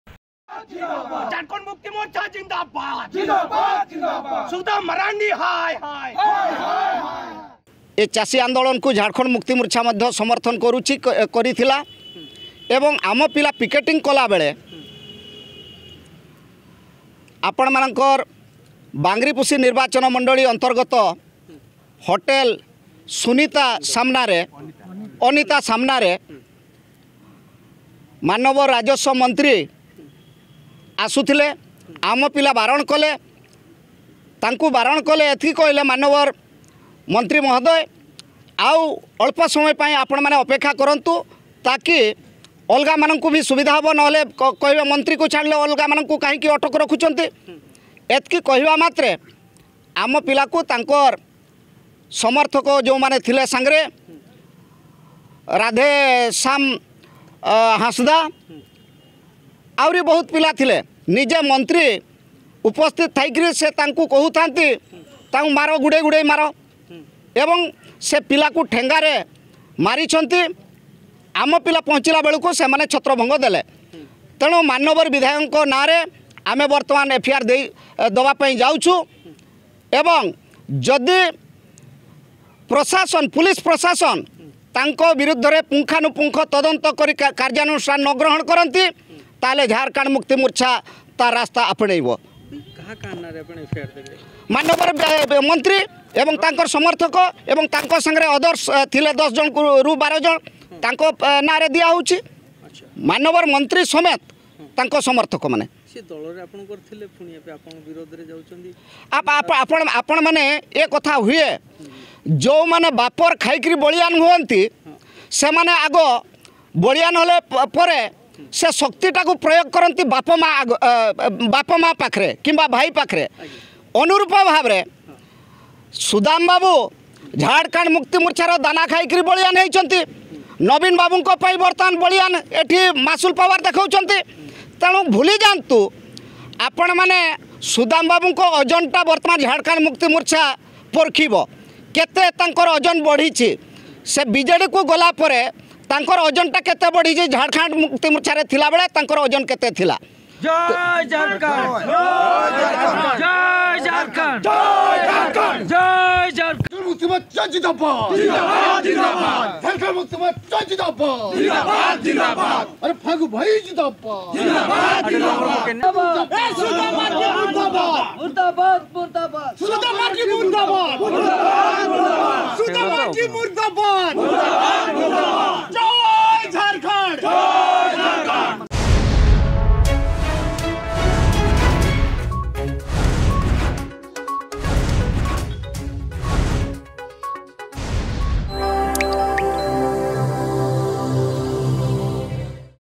ଝାଡ଼ଖଣ୍ଡ ମୁକ୍ତିମୋର୍ଚ୍ଚାର କର୍ମୀଙ୍କୁ ଆକ୍ରମଣ
ଝାଡ଼ଖଣ୍ଡ ମୁକ୍ତିମୋର୍ଚ୍ଚାର କର୍ମୀଙ୍କୁ ଆକ୍ରମଣ ନେଇ ରାଜସମନ୍ତ୍ରୀଙ୍କ ସୁଦାମ ମରାଣ୍ଡୀଙ୍କ ବିରୁଦ୍ଧରେ ବାଙ୍ଗିରିପୋଷି ଥାନାରେ ଅଭିଯୋଗ
ଅଧିକ ଖବର ପାଇଁ ଡ଼ାଉନଲୋଡ କରନ୍ତୁ “Argus News” ଆପ୍